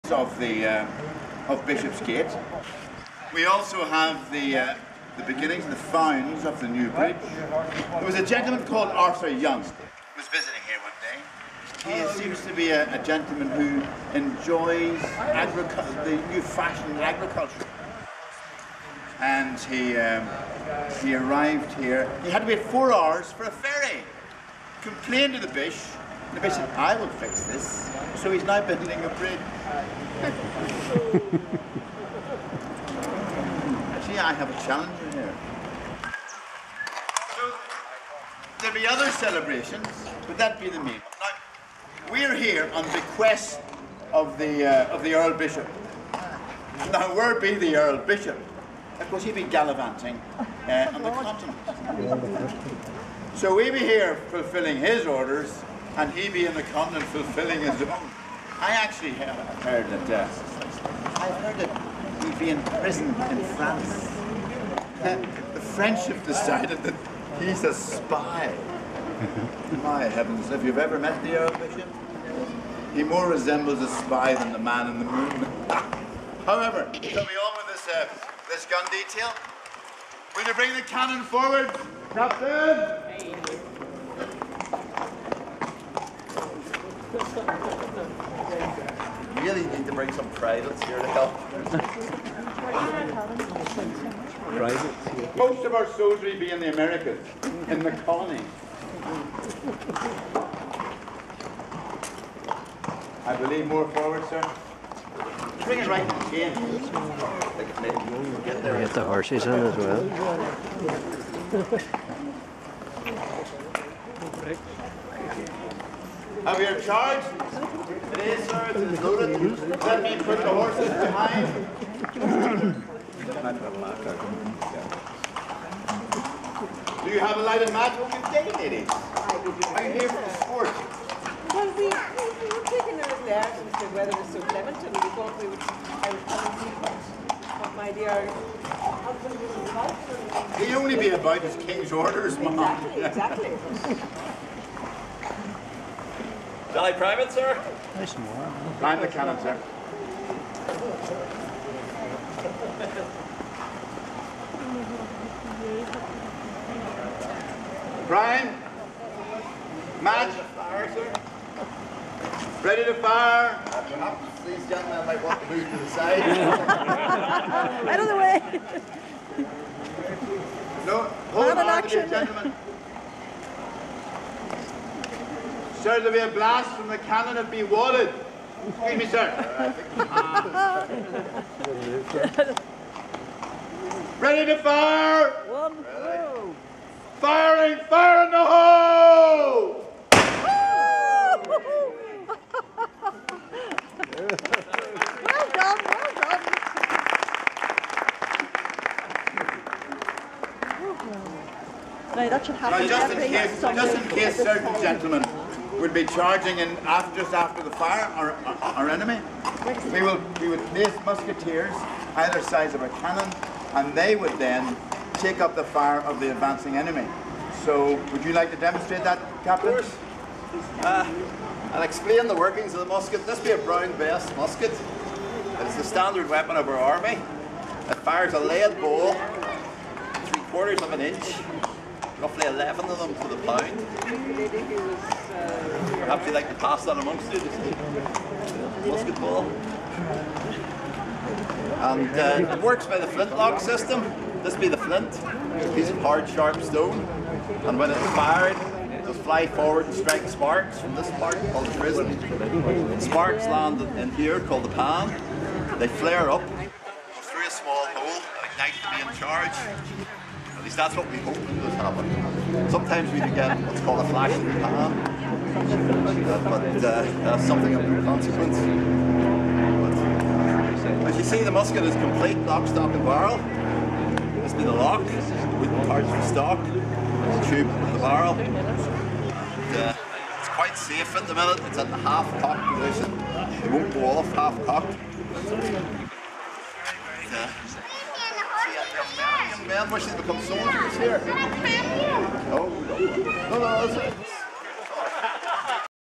St Columb's Cathedral Heralds the Start of National Heritage week
St Columb's Cathedral with National Heritage Week
Heralding the commencement of National Heritage Week the loading and firing of an ancient cannon was re-enacted dating from 1780's  at the Gates of St Columb's Cathedral, L'Derry on Sunday 19 August 2011.
This was repeated on the hour from 2pm - 4pm and visitors were also invited to the Cathedral to look at the treasures and also be entertained by The Dean, Very Rev. Dr. William Morton who played a variety of music on the Cathedral Organ